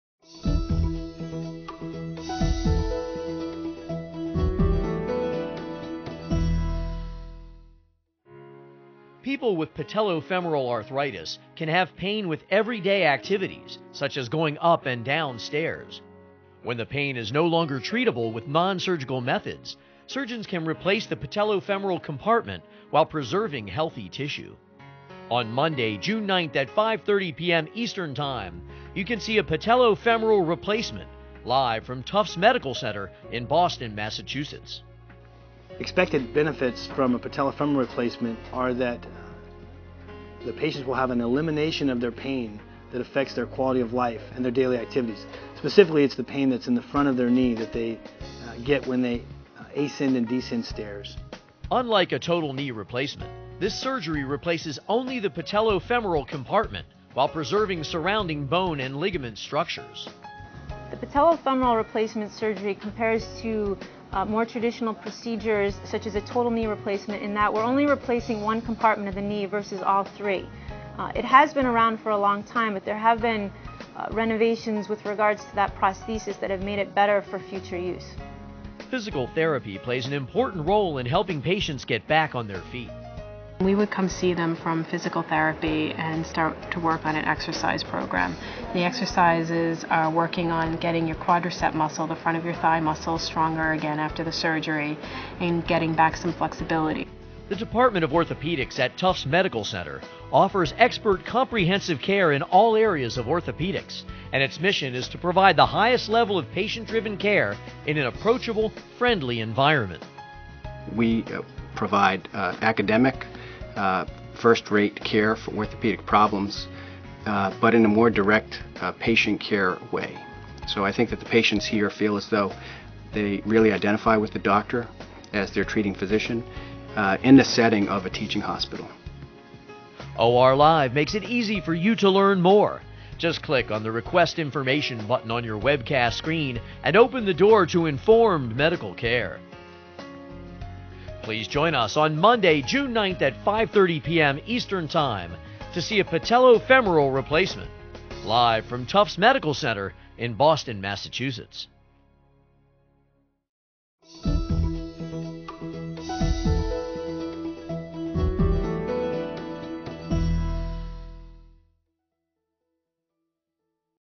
Patello Femoral Replacement Surgery
Tufts Medical Center and OR-Live will conduct a live Webcast of a patello femoral replacement operation

 Patello Femoral Replacement is performed to treat osteoarthritis affecting the back of the kneecap. Patients who are candidates for this procedure usually experience pain when crouching, going up or down stairs, walking up slopes and getting up from low chairs. If a patient's joint is mostly free from arthritis or displays minimal symptoms, Patello Femoral Replacement can provide pain relief and improve mobility. The procedure replaces the back of the patella and the front of the femur (thigh bone), and still allows patients to undergo total knee replacement at a later date if necessary.